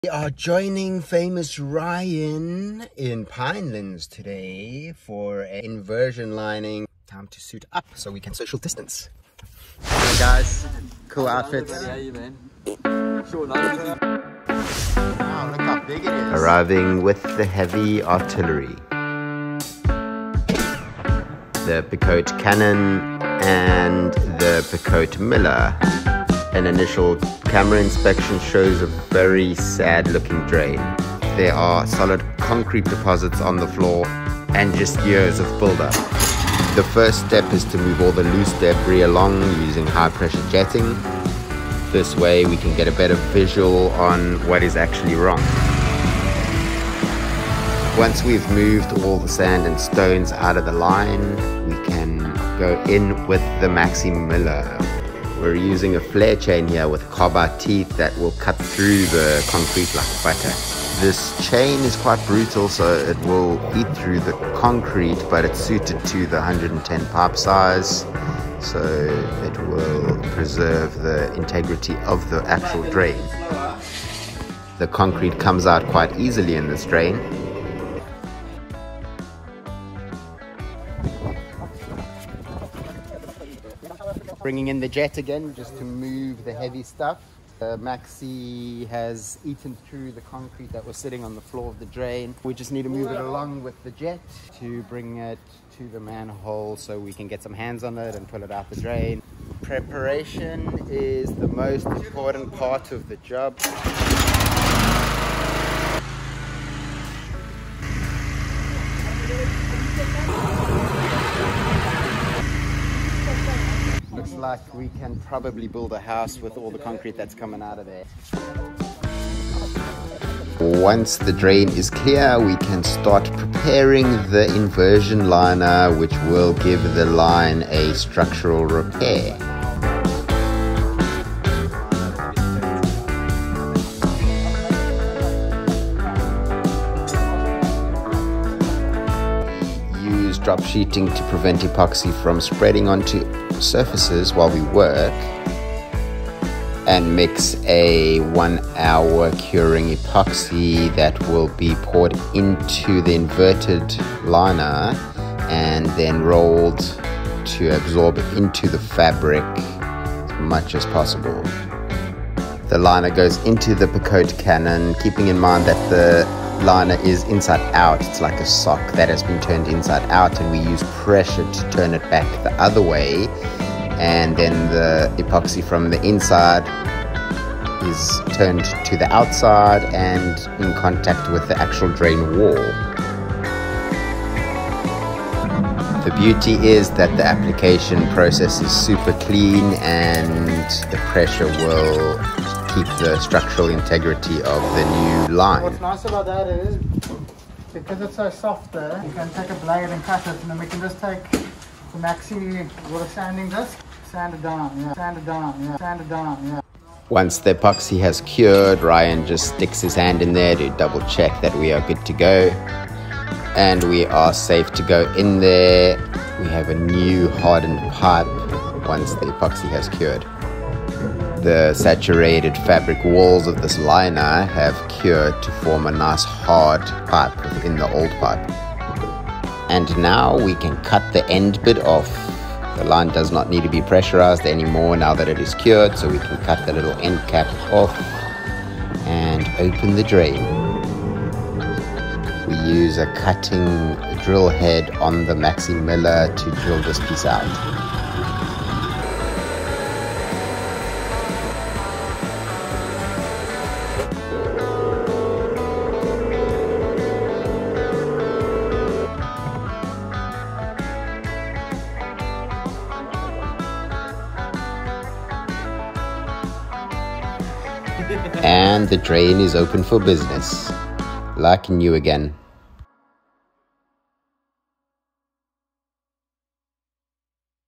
Inversion Drain Lining with Picote Cannon after Concrete removal
Joining team @plumvac_projects_services to cut dried concrete out a sewer line followed by a structural inversion lining to make the drain “Great Again”. It’s always fun working with great tools like the picote range. Using the minimiller , maximiller , picotecannon and epoxylining to clean and rehabilitate damaged drain lines. recycle reuse and makedrainsgreatagain footage edited on spliceapp from draincamera and shotoniphone